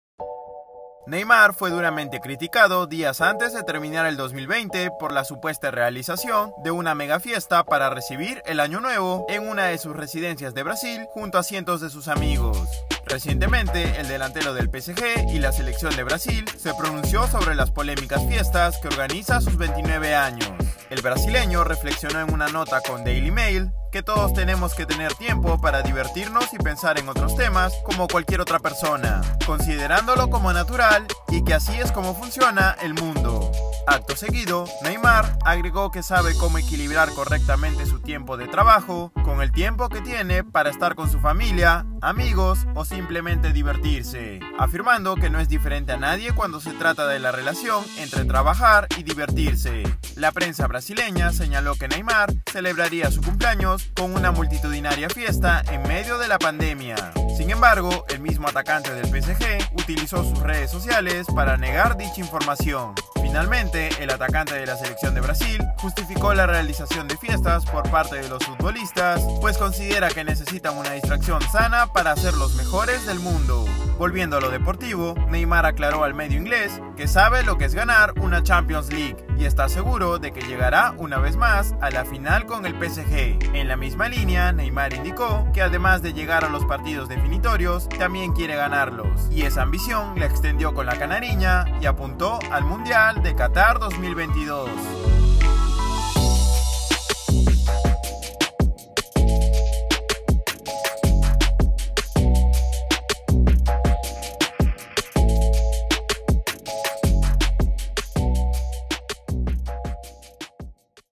Neymar sobre sus polémicas fiestas: “Tenemos que tener tiempo para divertirnos”
Delantero brindó una entrevista a ‘DailyMail’ en la que habló sobre sus multitudinarios eventos con cientos de amigos en medio de la pandemia. 'Ney' es conocido por hacer fiestas memorables en donde siempre es motivo de conversación.